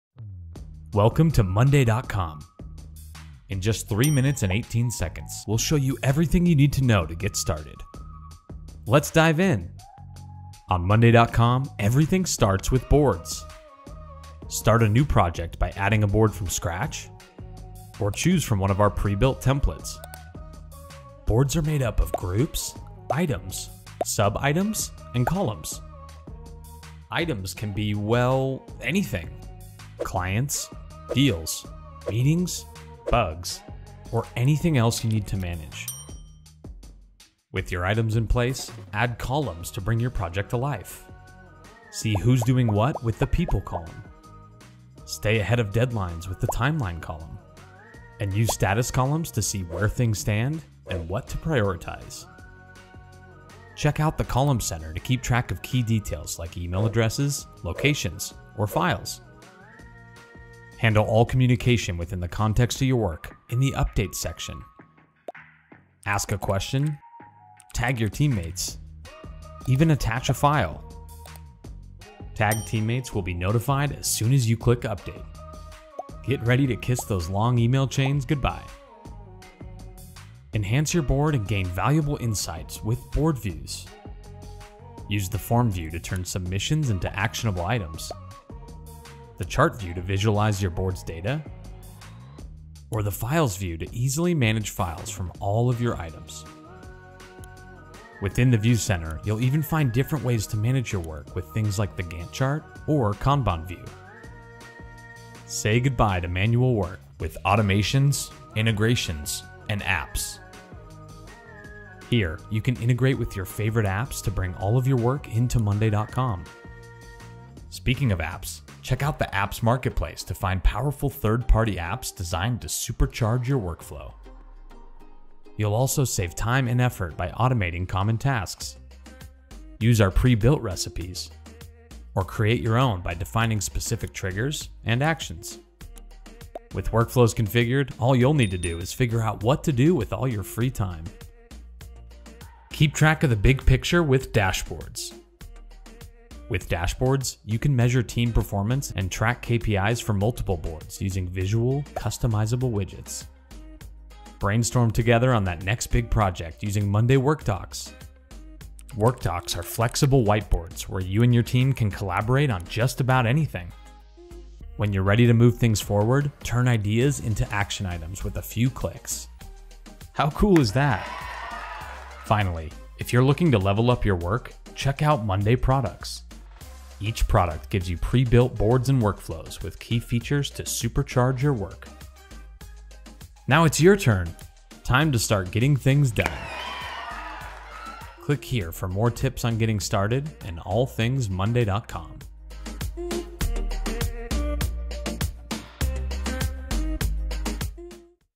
먼데이닷컴 사용법: 3분만에 시작하기 🚀
먼데이닷컴을 처음 시작하는 분들을 위해 준비했습니다 😇
영상을 통해 먼데이닷컴을 빠르게 익히고, 여러분의 워크플로우를 쉽게 구성해 보세요!

00:15 보드 추가하기
00:19 보드 템플릿에서 시작하기
00:24 보드 구조 설명
00:40 컬럼 추가하기
01:01 업데이트
01:21 뷰(보기) 타입
01:47 자동화, 연동, 앱
02:23 대시보드
02:46 Workdocs

프로젝트관리툴 먼데이닷컴이 더 궁금하신가요?

국내 유일 먼데이닷컴 플래티넘 파트너사 IFIED ✨
먼데이닷컴 관련 궁금한 사항은 아이파이드로 문의부탁드립니다.